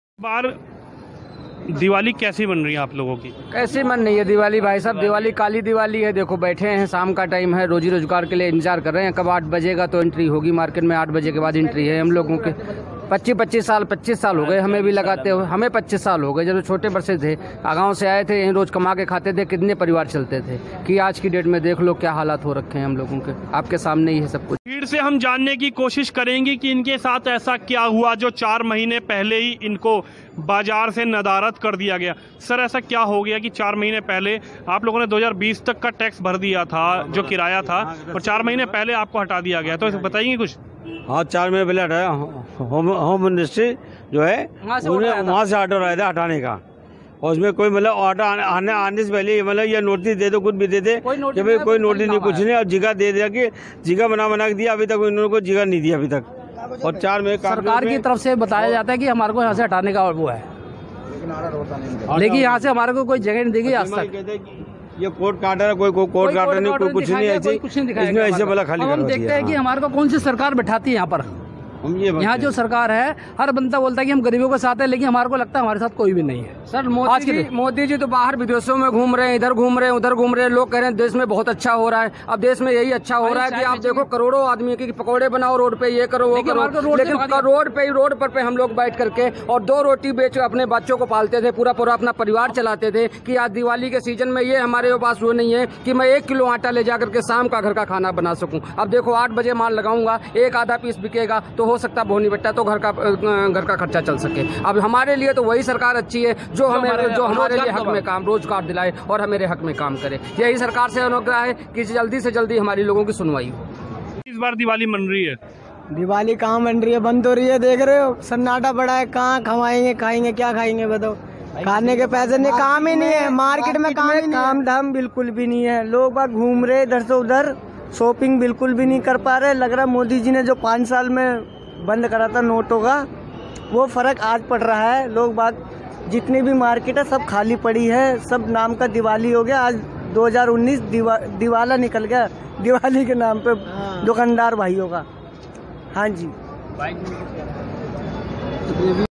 दिवाली पर दुकानदार क्यों हैं नाराज़
दिल्ली के करोल बाग़ में दुकानदारों की दिवाली पर क्या कहना है आप ज़रूर देखिये ...